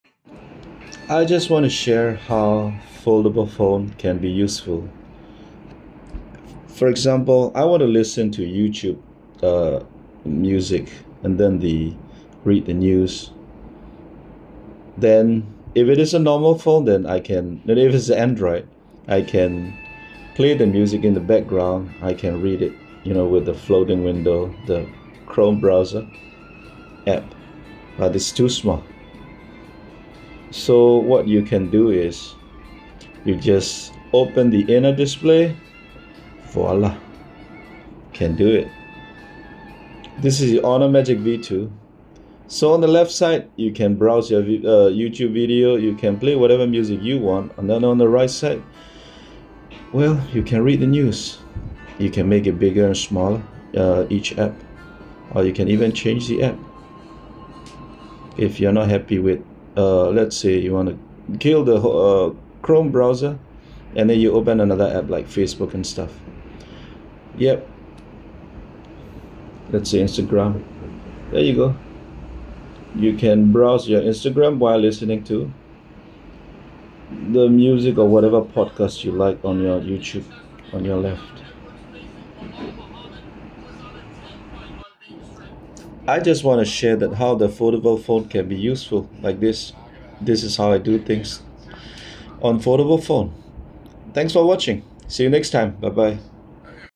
Honor Magic V2 Best Foldable Phone - Split screen
How to utilize bigger inner display with split screen apps.